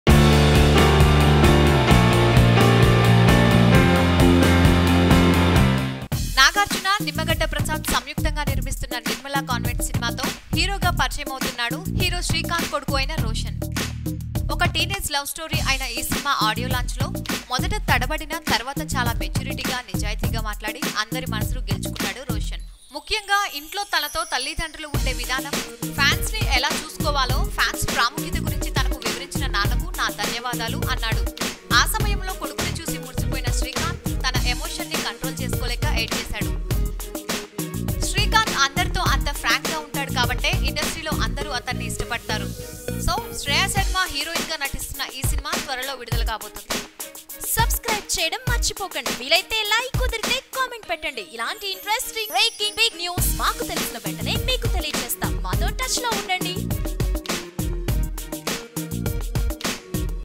Roshan Speech REPORT at Nirmala Convent Movie Audio Launch | Akkinni Nagarjuna | Srikanth | Ooha
Srikanth Son Roshan Speech REPORT at Nirmala Convent Telugu Movie Audio Launch by Top Telugu TV. Nirmala Convent Movie ft. Roshan, Shriya Sharma and is produced by Akkineni Nagarjuna and Nimmagadda Prasad.

Please Leave Your Comment Here